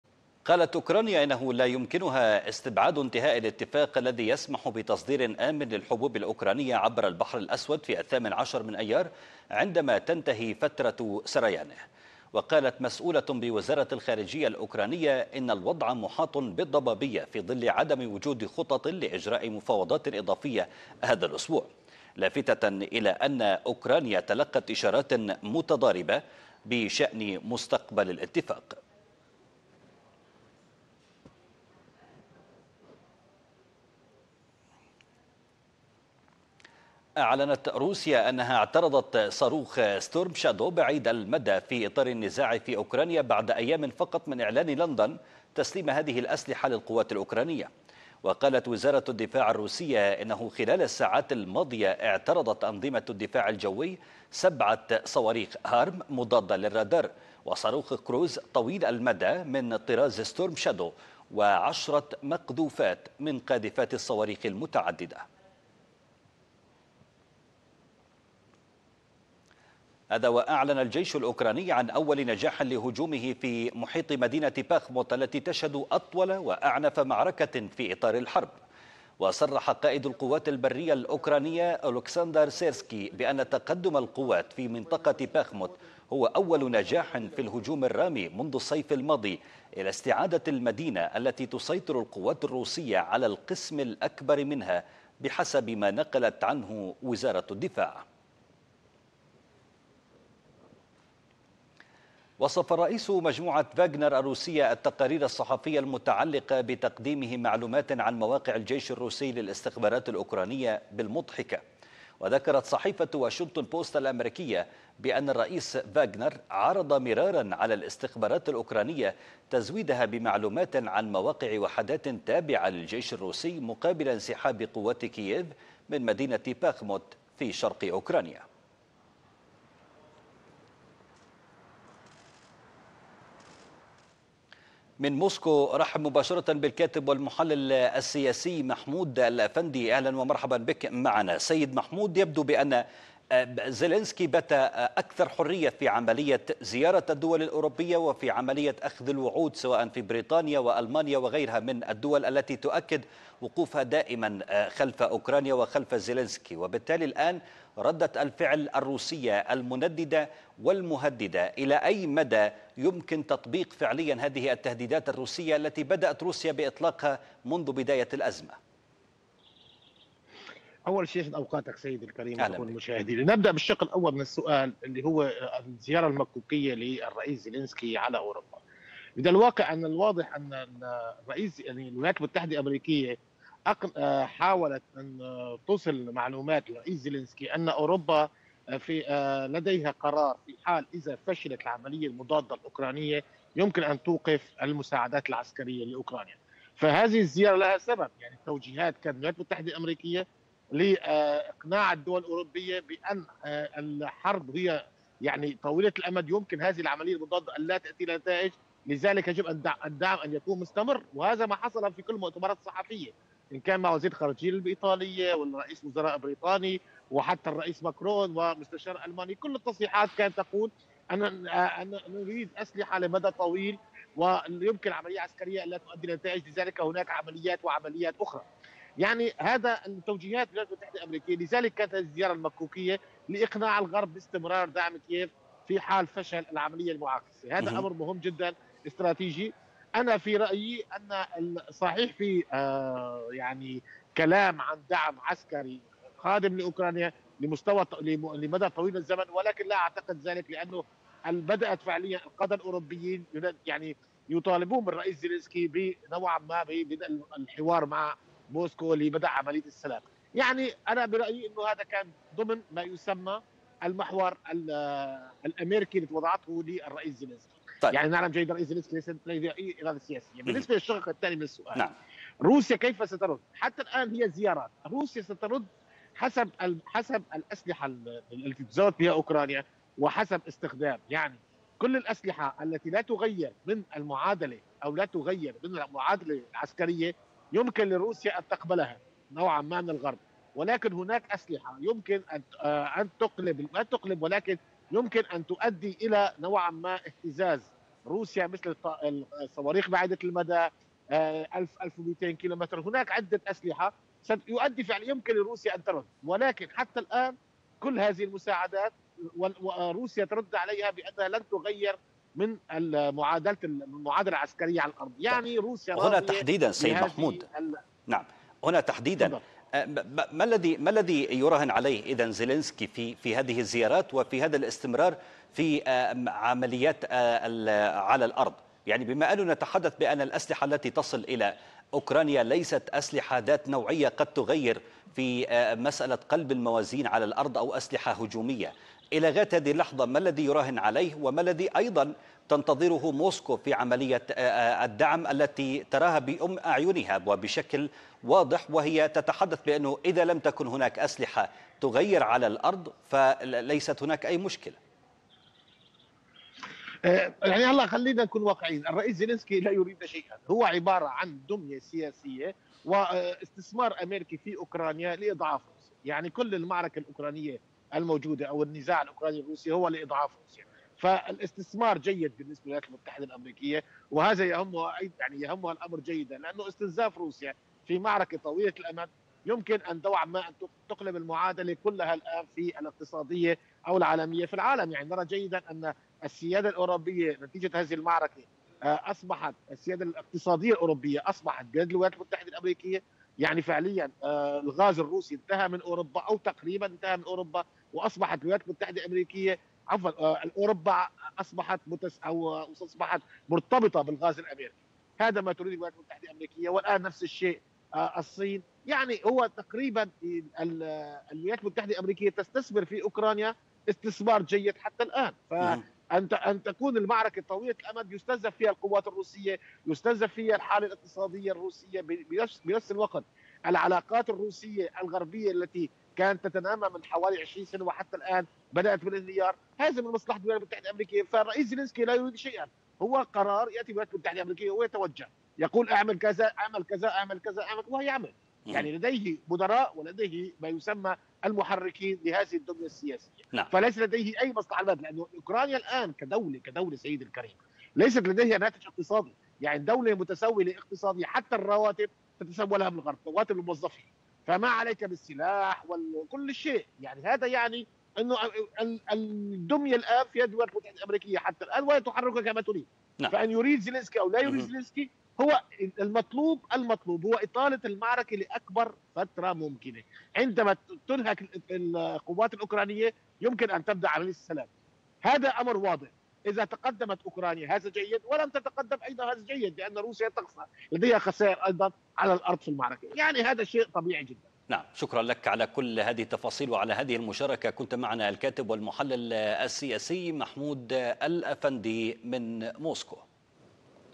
السادسة | أوكرانيا تعلن عن أول نجاح لهجومها في محيط باخموت
أعلن الجيش الأوكراني عن أول نجاح لهجومه في محيط مدينة باخموت التي تشهد أطول وأعنف معركة في إطار الحرب.
وصرح قائد القوات البرية الأوكرانية أولكسندر سيرسكي أن تقدم القوات في منطقة باخموت هو أول نجاح في الهجوم الرامي منذ الصيف الماضي، إلى استعادة المدينة التي تسيطر القوات الروسية على القسم الأكبر منها، بحسب ما نقلت عنه وزارة الدفاع.

الكاتب والمحلل السياسي محمود الأفندي

ـــــــــــــــــــــــــــــــــــــــــــــــــــــــــ
ـــــــــــــــــــــــــــــــــــــــــــــــــــــــــ
قناة المملكة
المملكة
AlMamlakaTV
Al Mamlaka
أخبار الأردن
أخبار العالم
ـــــــــــــــــــــــــــــــــــــــــــــــــــــــــ
قناة المملكة